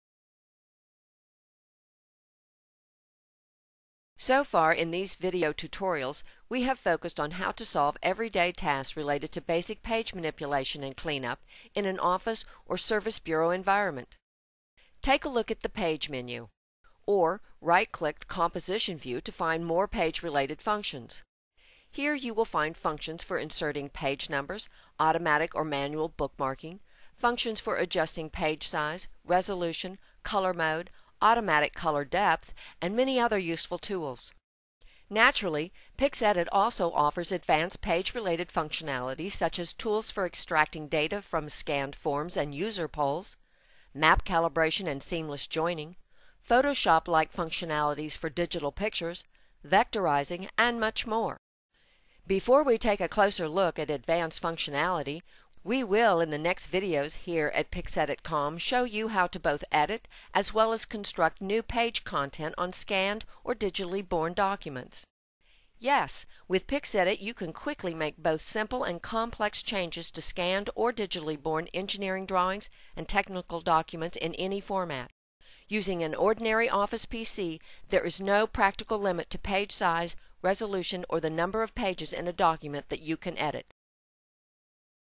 27 More page functions in PixEdit for Scanned PDF/TIFF/JPG files.
Here we'll look at the page menu with functions for inserting page numbers, automatic or manual bookmarking for PDF files, adjusting page size, resolution (dpi), color mode, automatic color depth and many other useful tools for amending your scanned image files.  for further information.